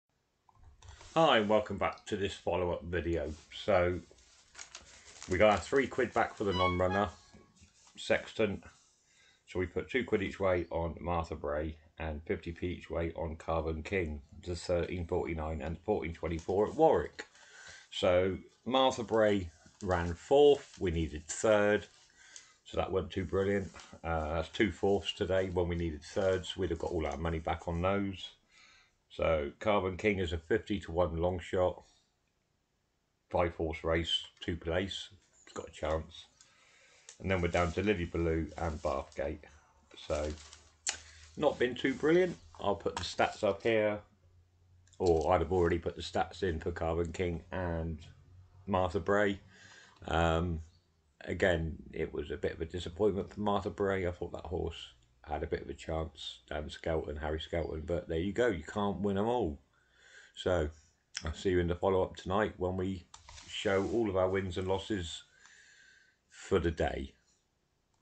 13/1/2024 HORSE RACING BETS PLACED (UPDATE)
a quick into to my betting hobby, some win some place most lose lol. bet small lose small bet big lose big. keep your bets small and within your means. don't bet to make money, you will lose more in desperation